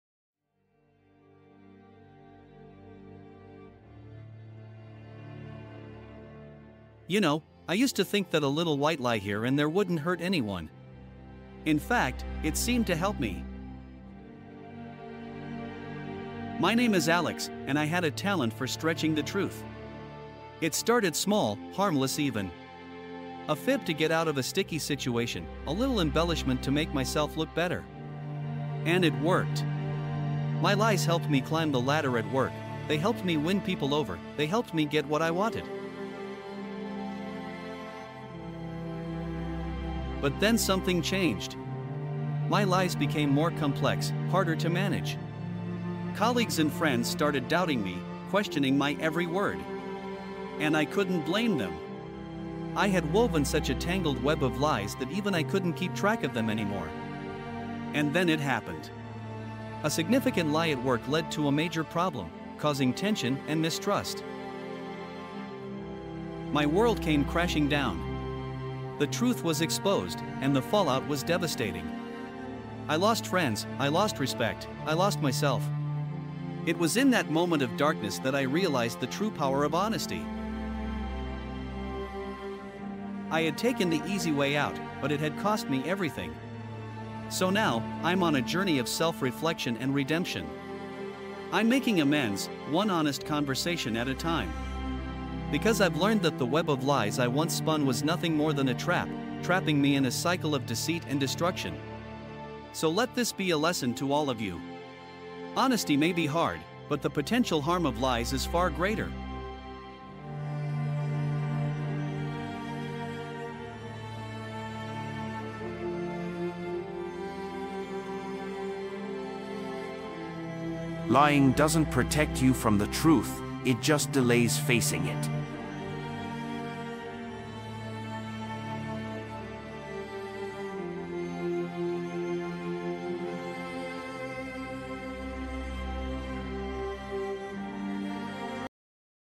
Journey of Deception | True Consequences of Dishonesty | Motivation video for who walk alone
**Description:** 🕸️ Discover the gripping tale of deception and truth in "The Web of Lies." This narrative explores the intricate and often unforeseen consequences of dishonesty. Watch as the protagonist, Alex, navigates a complex world of lies, only to find that truth is the ultimate key to freedom and integrity.

- The escalation of small lies into life-altering deceptions.
- The impact of dishonesty on personal and professional relationships.
- The psychological journey of the liar and the lied to.
- The ultimate revelation and the road to redemption.

🌟 **Why Watch "The Web of Lies"?**

- Engaging storytelling that mirrors real-life scenarios.
- Insights into the human psyche and the nature of truth.
- A powerful message about the importance of honesty and integrity.

- Anyone intrigued by psychological dramas.
- Individuals interested in moral and ethical dilemmas.
- Fans of narrative-driven content that provokes thought and discussion.